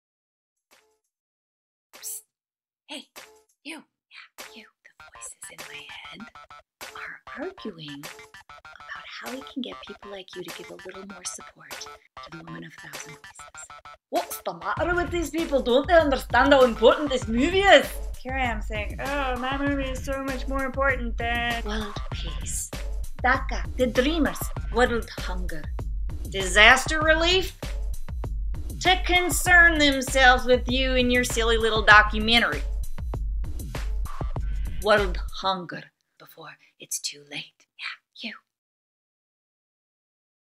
The Woman of a Thousand Voices: Before It's Too Late
With only days remaining on our Indiegogo campaign, Eliza Jane has a quick message for you about "The Woman of a Thousand Voices"...

Haven't supported yet, or want to provide more support?